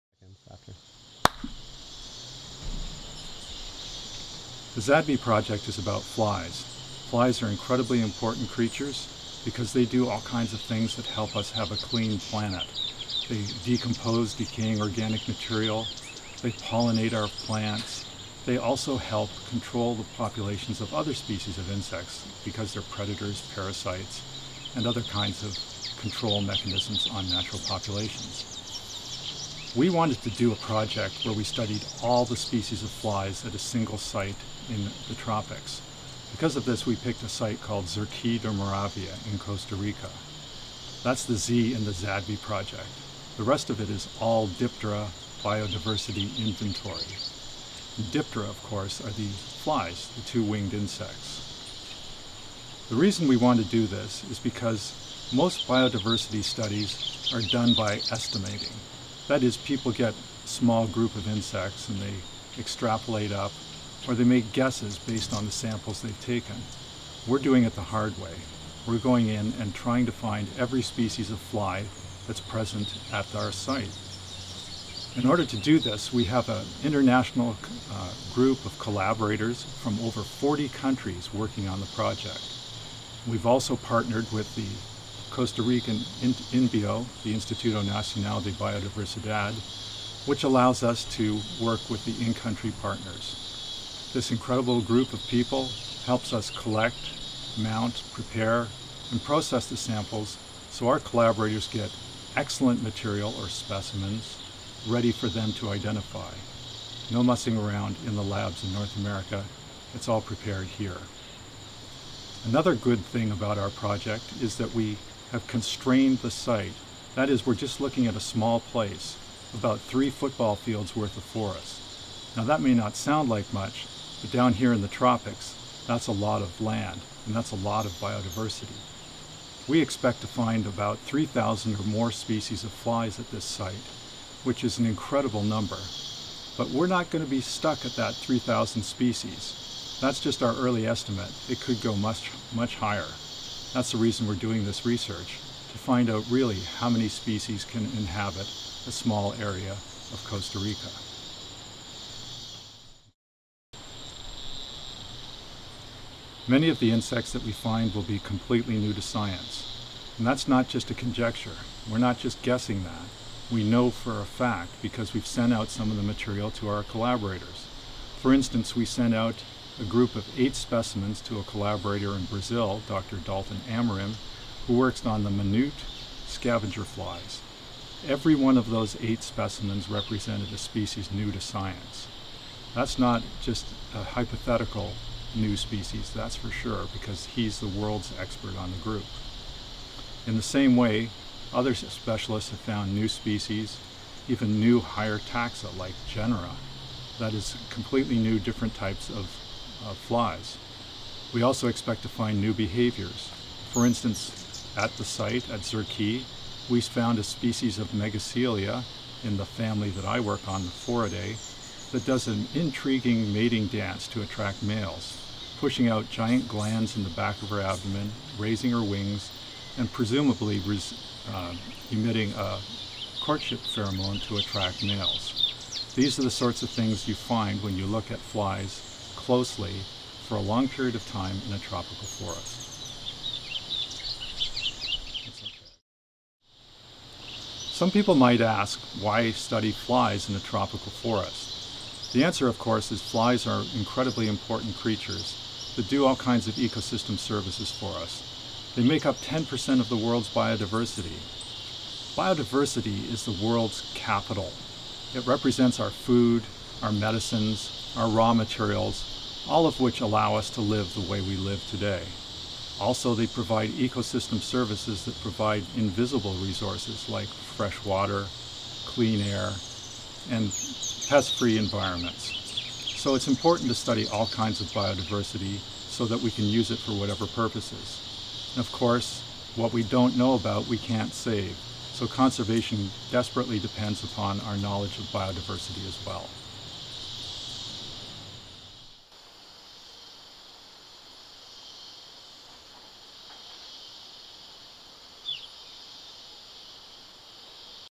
Dr. Brian Brown introduces ZADBI
A landmark research project using flies to estimate tropical biodiversity. Find out more about the NSF funded study in Costa Rica.

Dr. Brian Brown is curator of Entomology at the Natural History Museum of Los Angeles County.